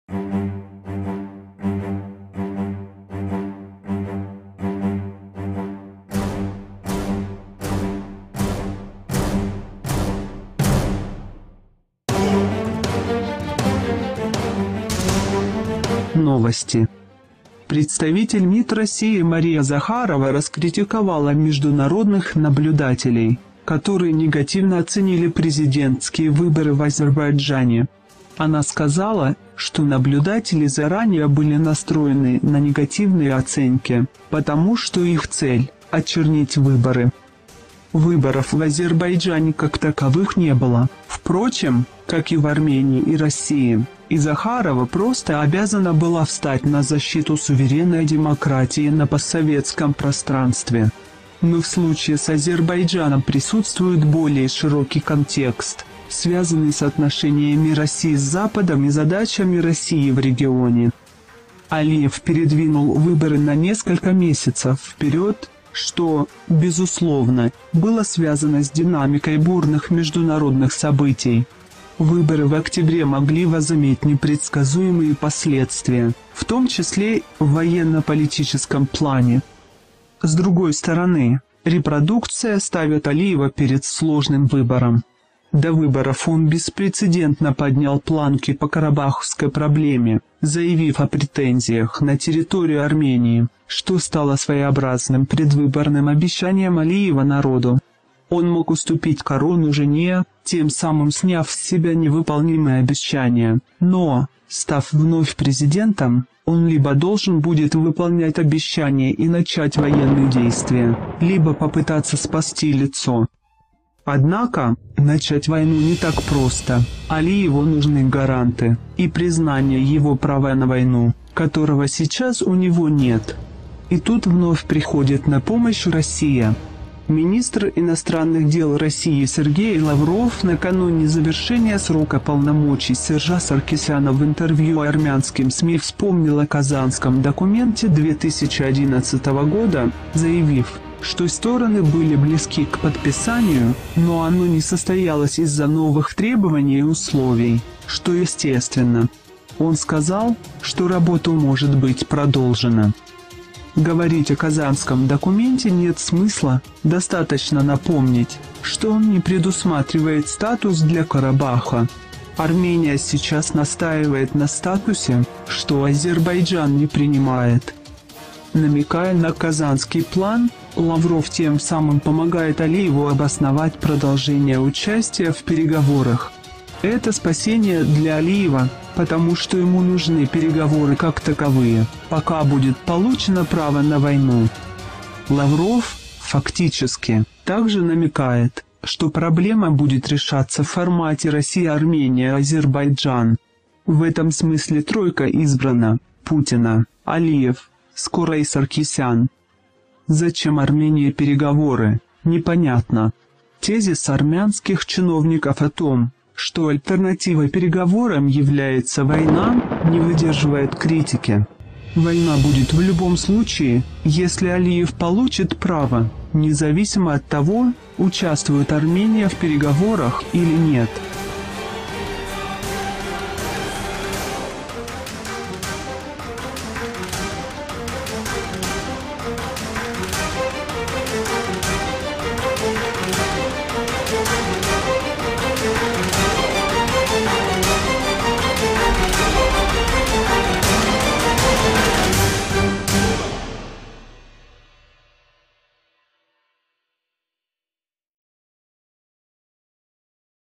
Россия помогла Азербайджану: Алиев будет ждать приказа
Представитель МИД России Мария Захарова раскритиковала международных наблюдателей, которые негативно оценили президентские выборы в Азербайджане. Она сказала, что наблюдатели заранее были настроены на негативные оценки, потому что их цель – очернить выборы.
Выборов в Азербайджане как таковых не было, впрочем, как и в Армении и России, и Захарова просто обязана была встать на защиту «суверенной демократии» на постсоветском пространстве. Но в случае с Азербайджаном присутствует более широкий контекст, связанный с отношениями России с Западом и задачами России в регионе.
Алиев передвинул выборы на несколько месяцев вперед, что, безусловно, было связано с динамикой бурных международных событий. Выборы в октябре могли возыметь непредсказуемые последствия, в том числе, в оенно-политическом плане.
С другой стороны, репродукция ставит Алиева перед сложным выбором. До выборов он беспрецедентно поднял планки по Карабахской проблеме, заявив о претензиях на территорию Армении, что стало своеобразным предвыборным обещанием Алиева народу. Он мог уступить «корону» жене, тем самым сняв с себя невыполнимые обещания, но, став вновь президентом, он либо должен будет выполнить обещания и начать военные действия, либо попытаться спасти лицо.
Однако, начать войну не так просто, Алиеву нужны гаранты и признание его права на войну, которого сейчас у него нет. И тут вновь приходит на помощь Россия. Министр иностранных дел РФ Сергей Лавров накануне завершения срока полномочий Сержа Саркисяна «в интервью армянским СМИ» вспомнил о Казанском документе 2011 года, заявив, что стороны были близки к подписанию, но оно не состоялось из-за новых требований и условий, что естественно. Он сказал, что работа может быть продолжена.
Говорить о Казанском документе нет смысла, достаточно напомнить, что он не предусматривает статус для Карабаха. Армения сейчас настаивает на статусе, что Азербайджан не принимает.
Намекая на Казанский план, Лавров тем самым помогает Алиеву обосновать продолжение участия в переговорах. Это спасение для Алиева, потому что ему нужны переговоры как таковые – пока будет получено право на ….
Лавров, фактически, также намекает, что проблема будет решаться в формате РФ-Армения-Азербайджан. В этом смысле «тройка» избрана – Путина, Алиев, скоро и Саркисян.
Зачем Армении переговоры, не понятно. Тезис армянских чиновников о том, что альтернативой переговорам является …., не выдерживает критики….